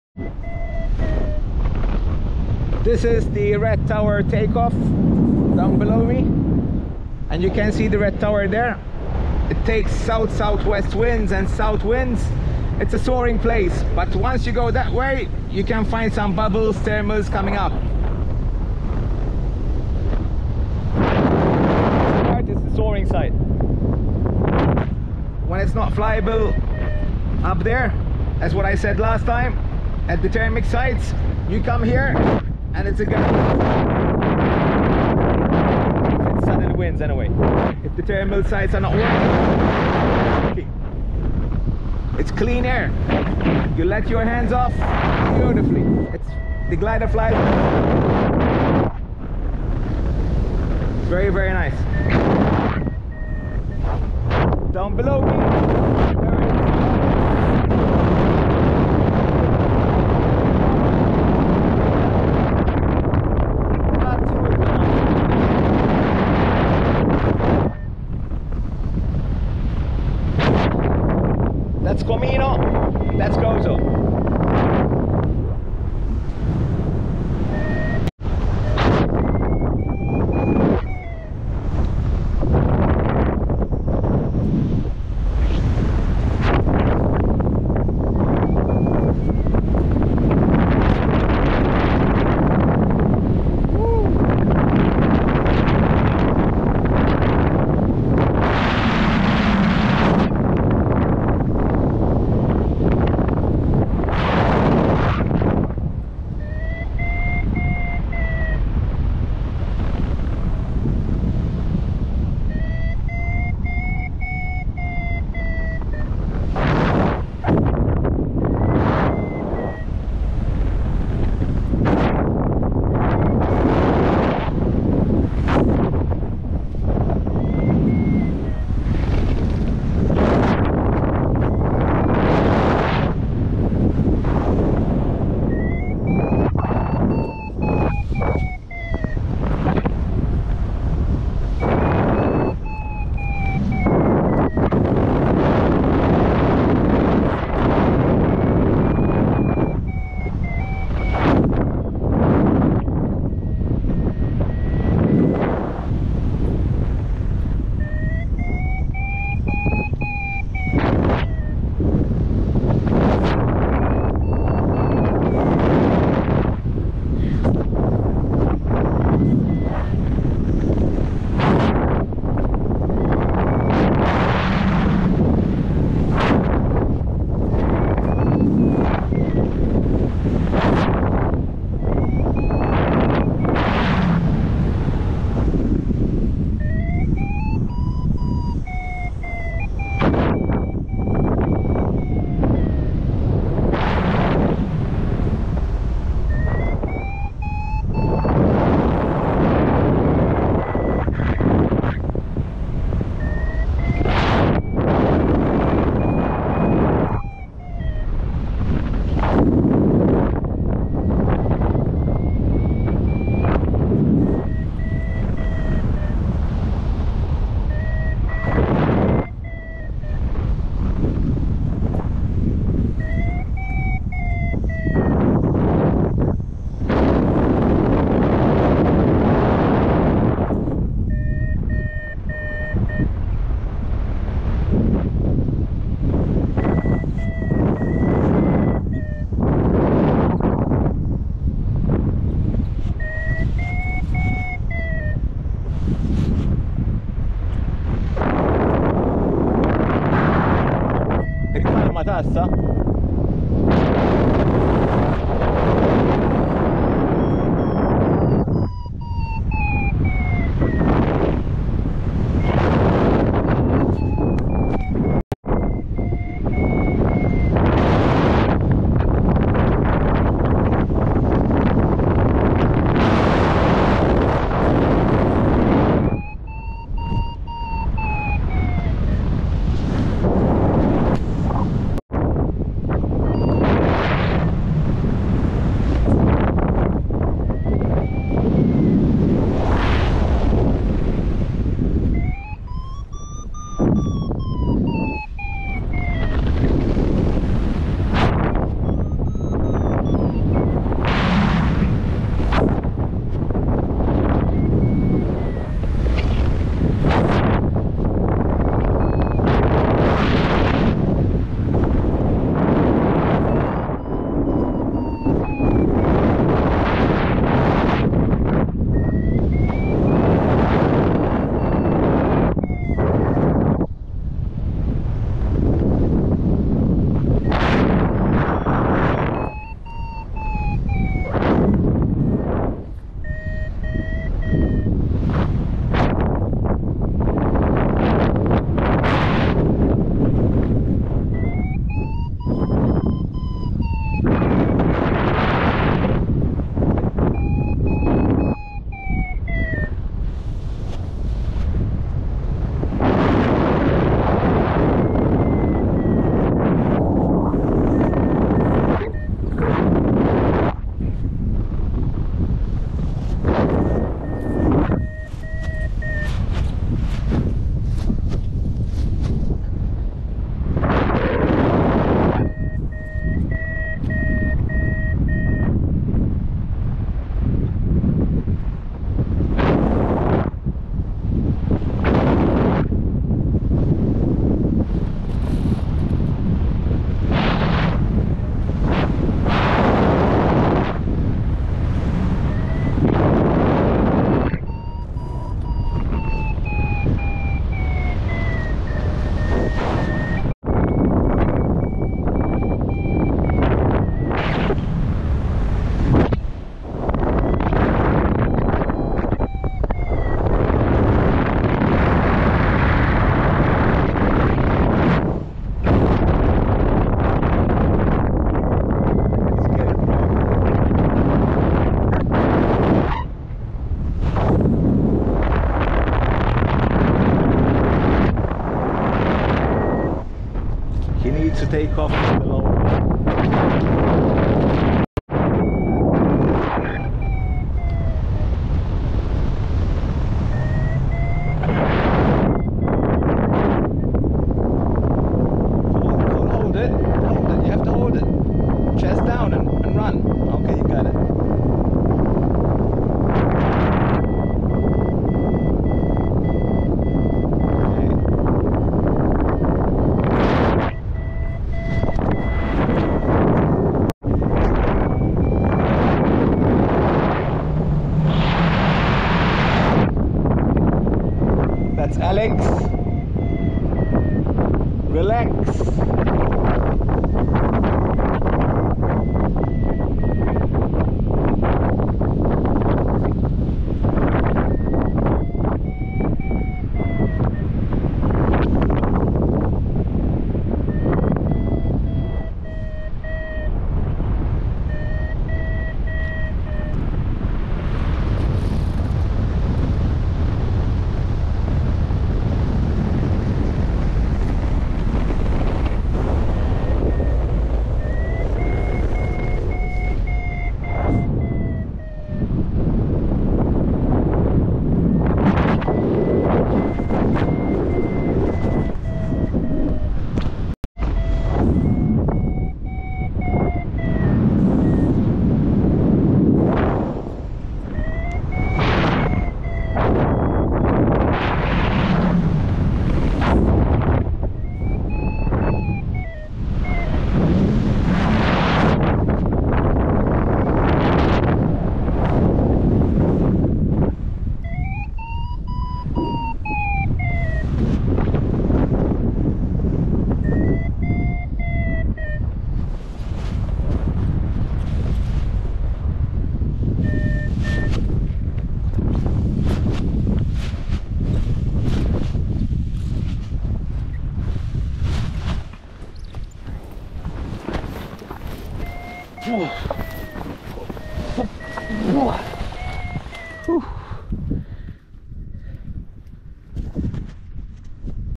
Paragliding Red Tower Malta /Sep 2024
The Take off was a bit on the strong side , i took of when it was 33 km/h then it was still gusty at some times.it was hard to land the glider, it had so much pull and power.